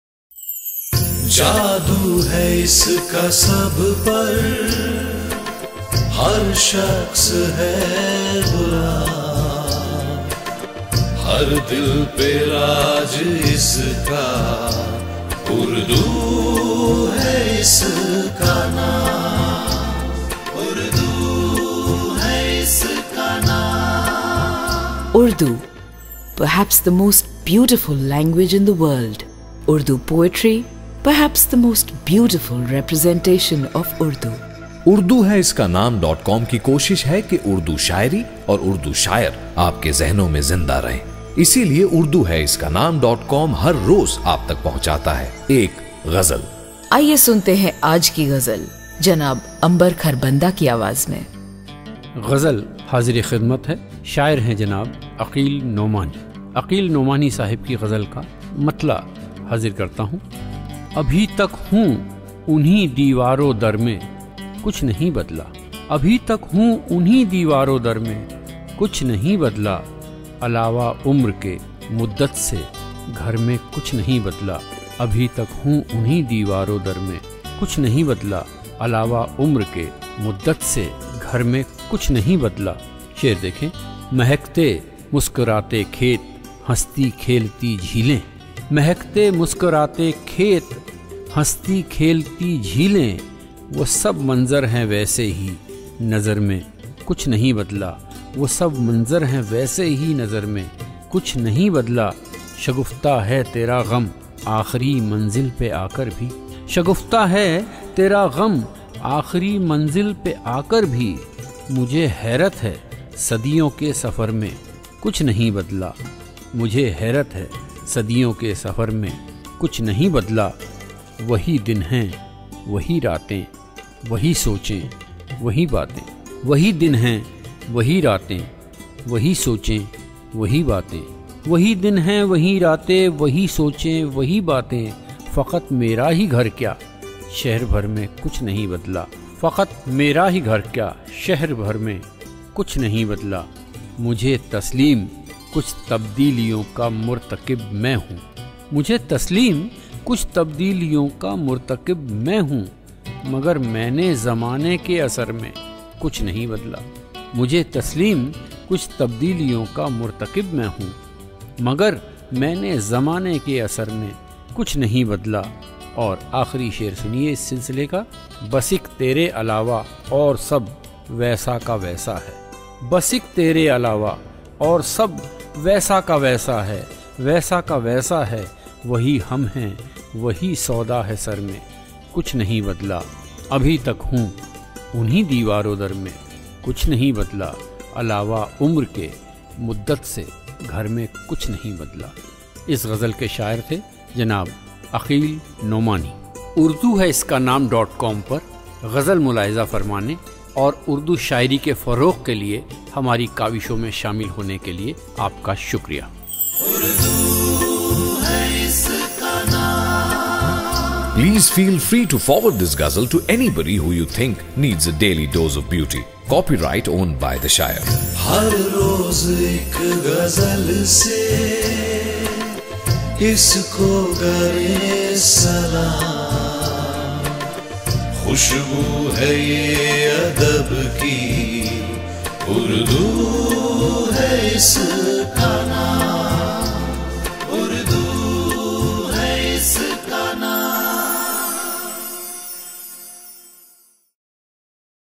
'Daily Ghazal' | 'Aqeel' Nomani | "Abhi Tak Hoon..."
As part of our initiative to present contemporary Shayars, we present today Janaab 'Aqeel' nomani's Ghazal, this Ghazal is taken from his  collection of urdu shayary 'Rahguzar',his other books are  'Parwaz ka Mausam' and 'Sarmaya', enjoy..."
'Ambar' Kharbanda
Urdu Hai Iska Naam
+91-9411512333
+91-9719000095